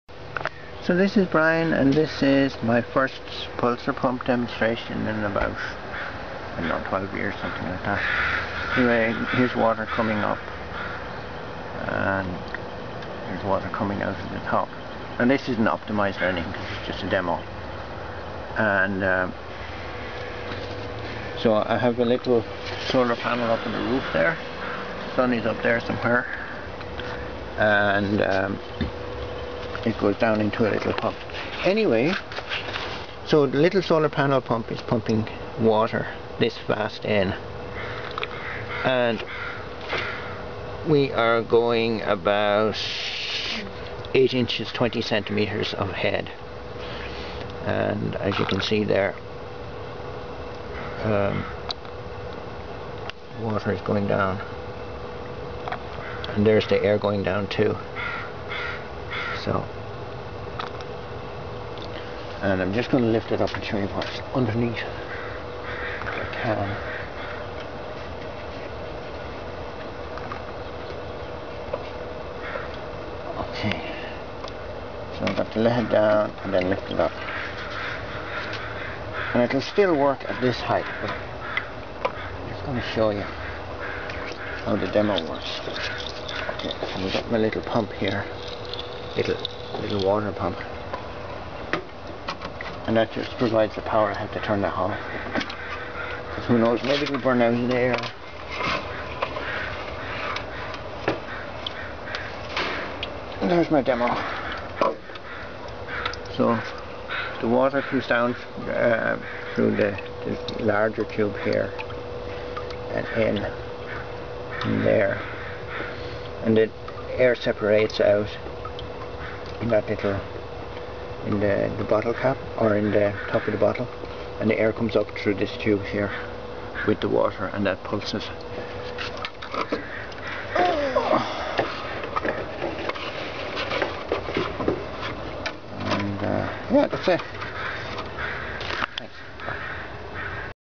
Tiny pulser pump "nano" demonstration and perhaps the world's smallest compressor!
This is my first pulser pump demo in over 10 years.  Even though this is tiny tiny, in real life it would produce enough low pressure air to run a pallet garden.   Why use a solar water pump to produce  compressed air?  (I hear you asking!).  I looked online and solar water pumps last a very long time in clean water (over a year)  and start and stop automatically AND the water immersion prevents overheating.  (That is a few reasons).  If you wanted to do this large scale, perhaps a bilge pump would do the job for you. The amount of compression you need depends on your individual set up.  You can produce just the right pressure with the bilge pump/tromp combination  but you will might have over pressure with a direct powered solar air compressor.  You need 1 psi for pallet gardens.  Over pressure is wasted.  (I have not yet tried a direct solar compressor on any long term use and they might work great).   Someone might like to try?  Brian